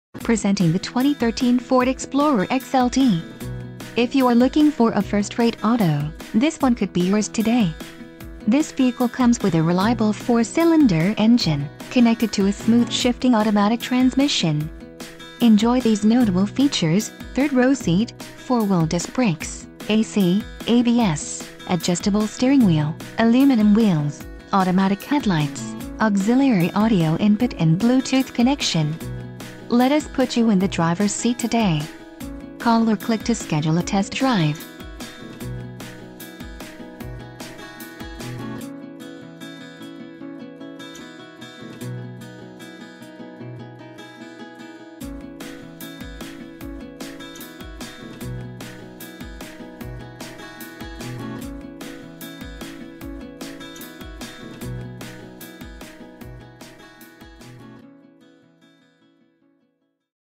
2013 Ford Explorer XLT - Jim Marsh Kia - Jim Marsh Kia, N...
Climb inside the 2013 Ford Explorer! Feature-packed and decked out! With fewer than 35,000 miles on the odometer, this 4 door sport utility vehicle prioritizes comfort, safety and convenience. Ford prioritized fit and finish as evidenced by: front and rear reading lights, front and rear air conditioning, and a split folding rear seat. Smooth gearshifts are achieved thanks to the efficient 4 cylinder engine, and for added security, dynamic Stability Control supplements the drivetrain. Our aim is to provide our customers with the best prices and service at all times. Stop by our dealership or give us a call for more information.